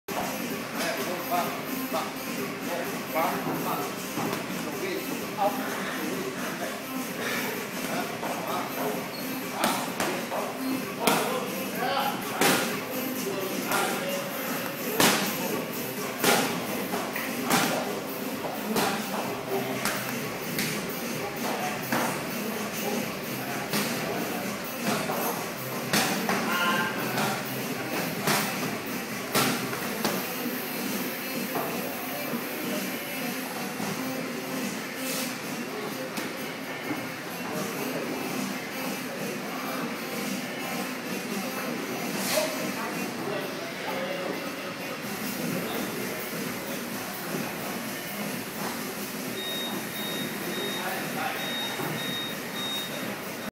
FightWorksAsia Advance Class
Instructor Nick working on Sparring with students.